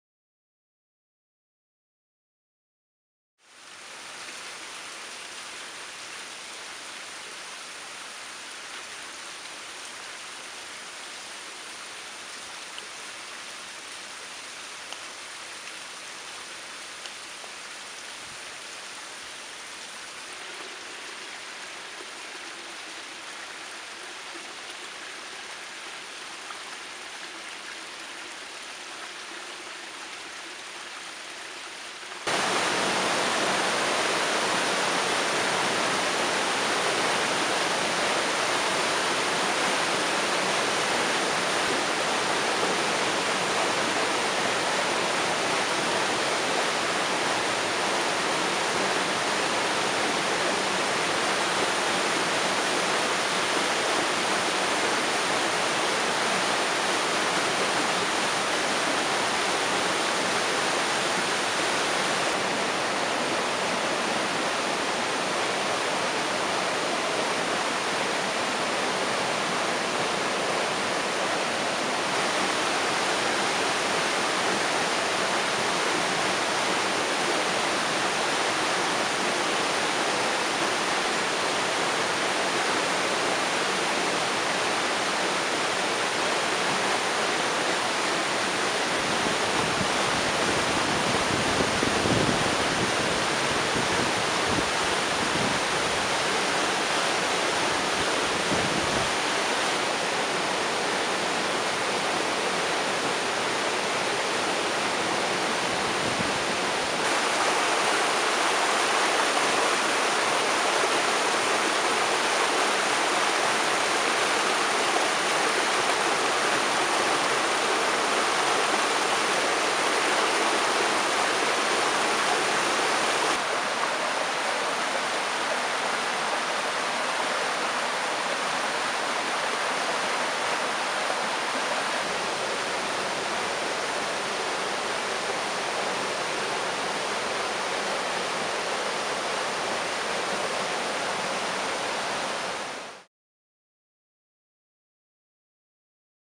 The Symmetrical Winter Waters of Tien Mu Creek - 20140118
Footage of Tien Mu Creek.  Symmetrical views...